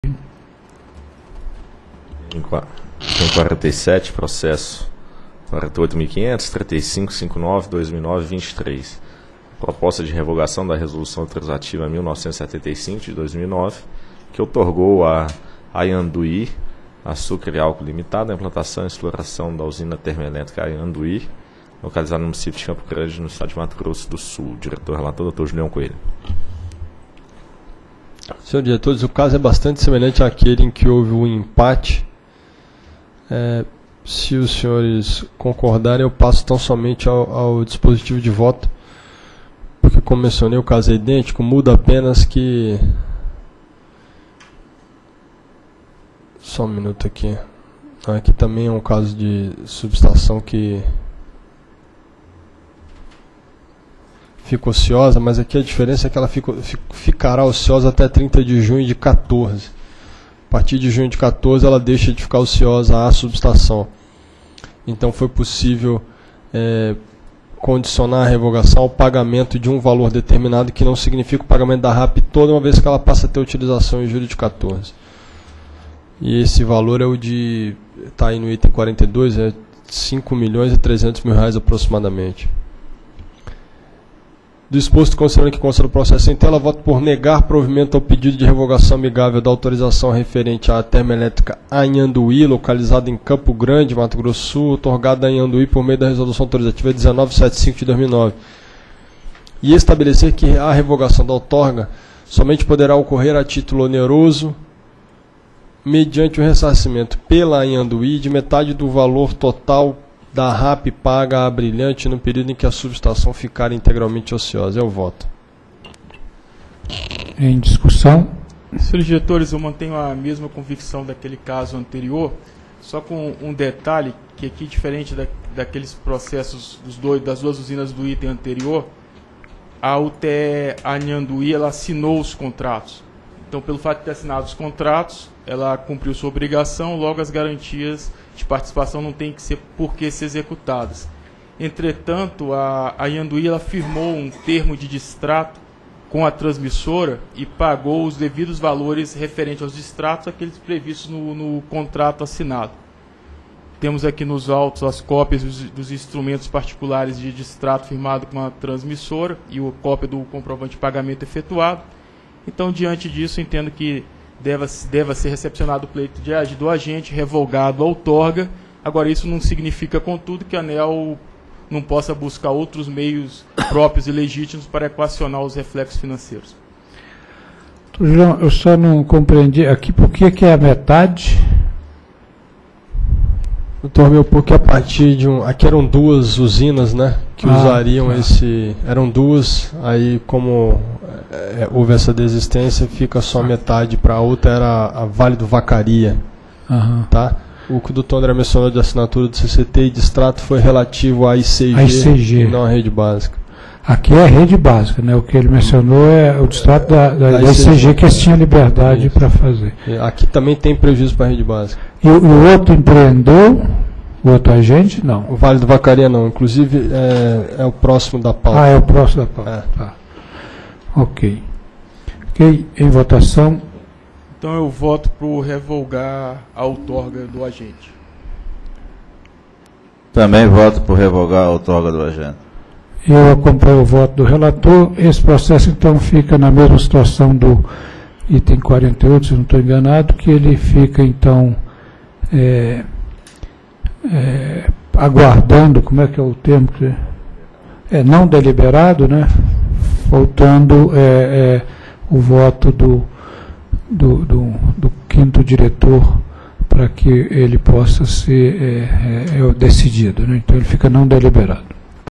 Item 47 - 26ª Reunião Pública Ordinária ANEEL 16/07/2013
Assunto: Proposta de revogação da Resolução Autorizativa nº 1.975/2009, que outorgou à Anhanduí Açúcar e Álcool Ltda. a implantação e exploração da Usina Termelétrica Anhanduí, localizada no município de Campo Grande, no estado de Mato Grosso do Sul.  Área Responsável: Superintendência de Concessões e Autorizações de Geração -- SCG.